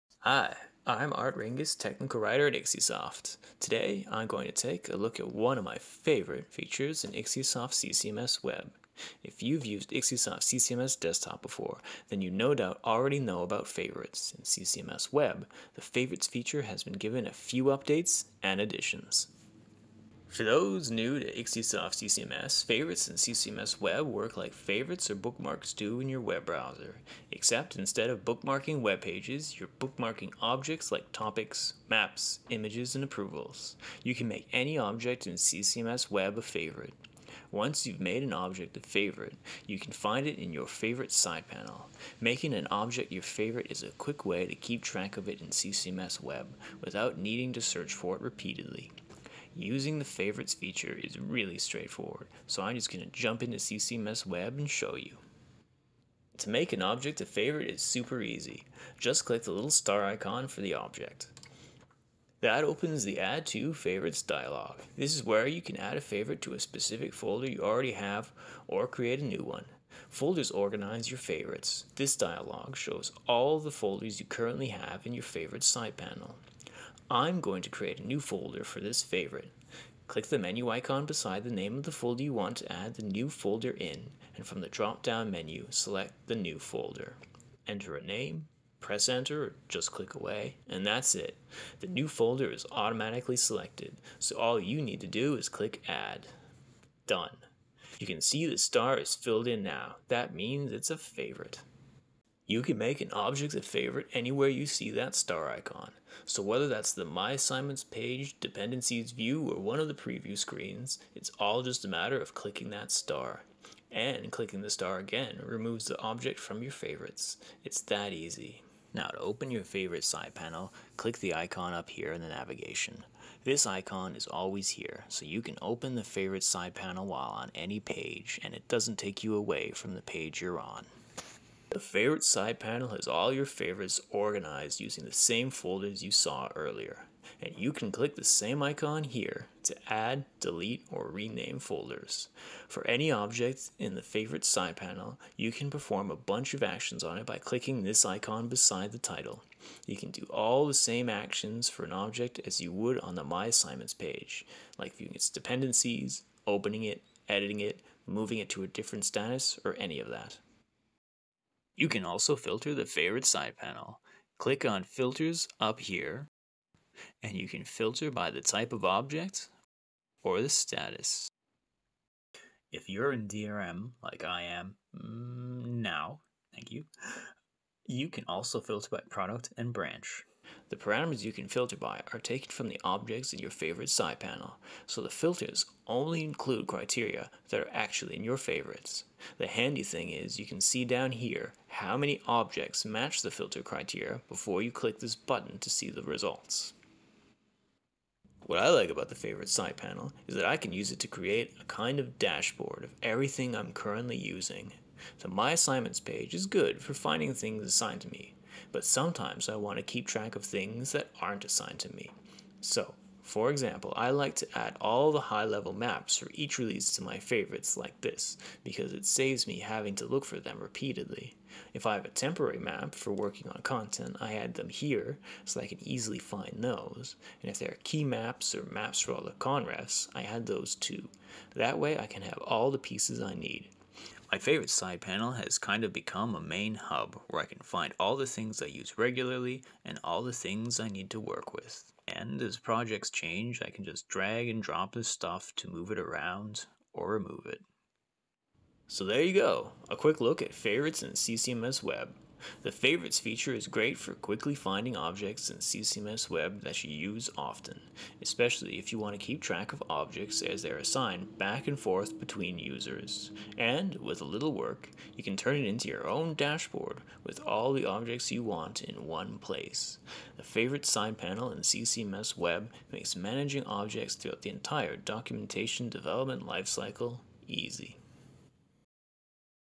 MadCap IXIA CCMS Features - Favorites in CCMS Web
In this video, Technical Writer Art Ringis explores the Favorites feature in CCMS Web. For those new to Madcap IXIA CCMS, Favorites in CCMS Web works like favorites or bookmarks do in your web browsers. Instead of bookmarking webpages, it allows bookmarking of objects like topics, maps, images, and approvals. Any object in CCMS Web can be made a favorite.